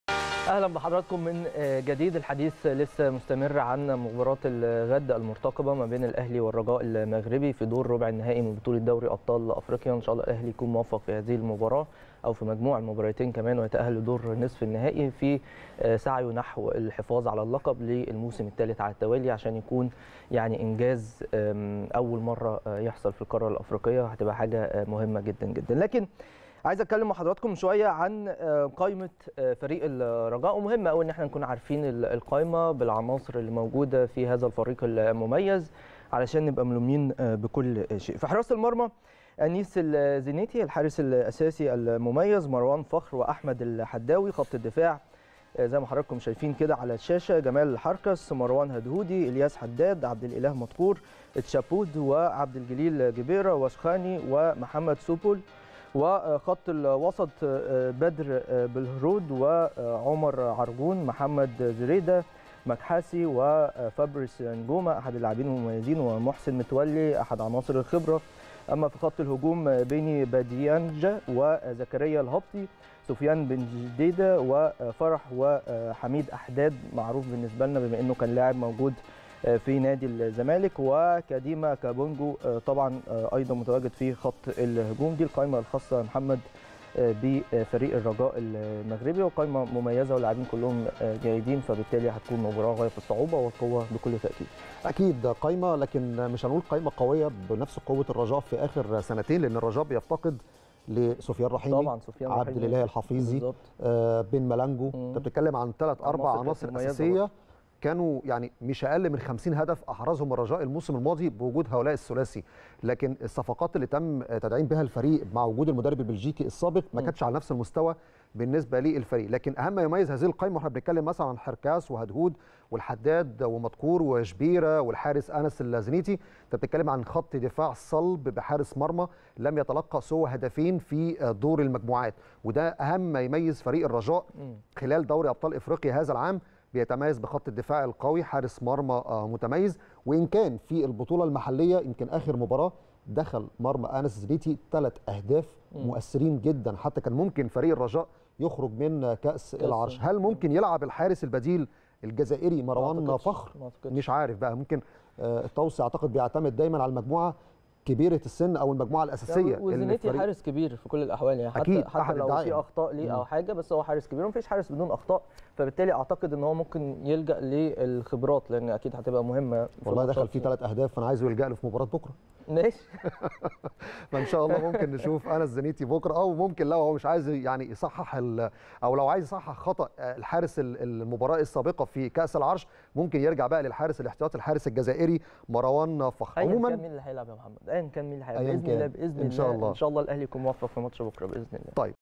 محمد سعيد وأمير هشام يستعرضان قائمة الرجاء المغربي لمباراة الأهلي غدا بدوري أبطال إفريقيا
محمد سعيد وأمير هشام يستعرضان قائمة الرجاء المغربي لمباراة الأهلي غدا بدوري أبطال إفريقيا | الأهلي في رمضان